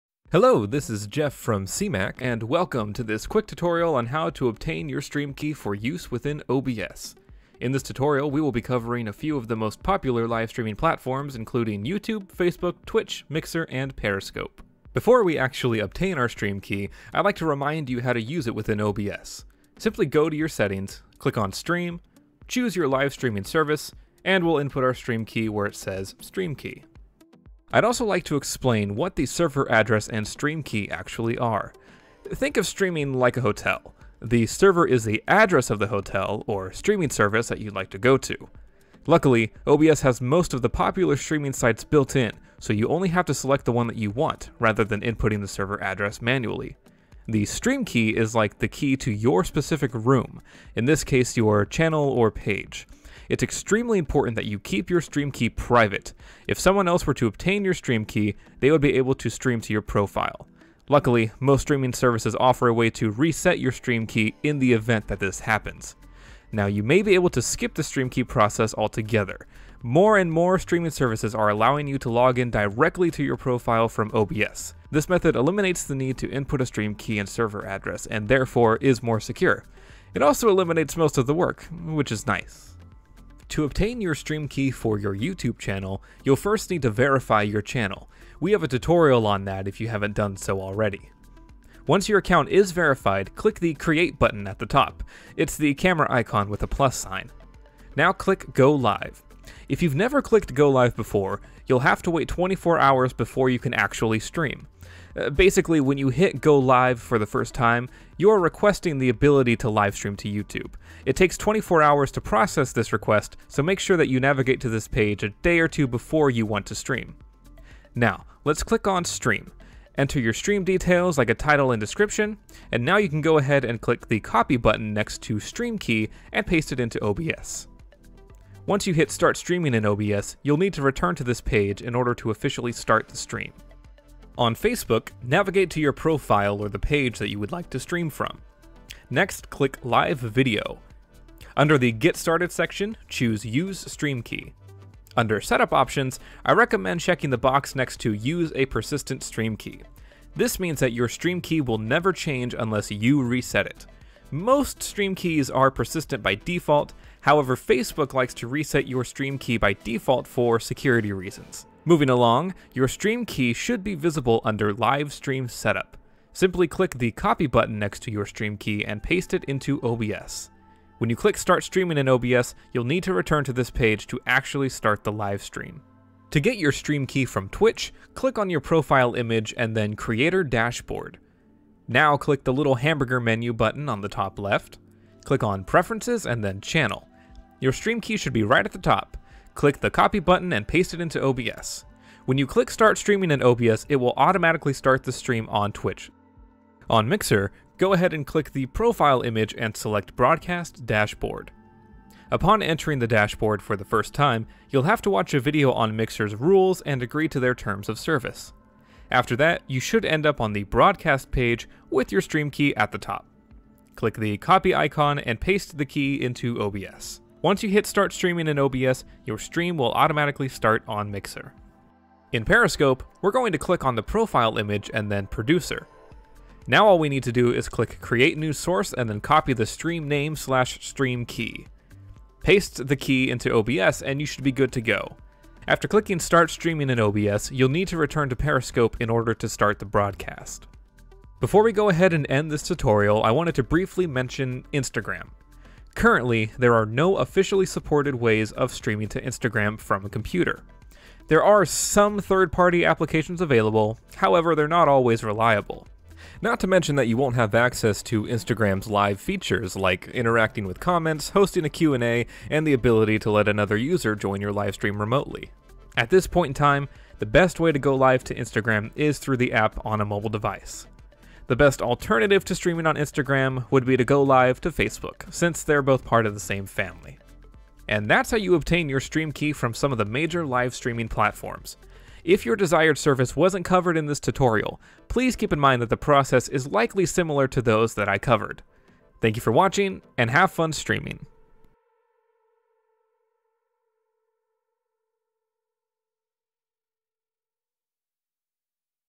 Where To Get Stream Keys For OBS (Tutorial)
Wanna know where to get streak keys for various platforms (YouTube, Facebook, Twitch, Mixer, Periscope) for live streaming in OBS? Jeff has you covered.

This video is from CMAC — the community media center for Fresno and Clovis, a non-profit organization whose mission is to empower voices in our community through media.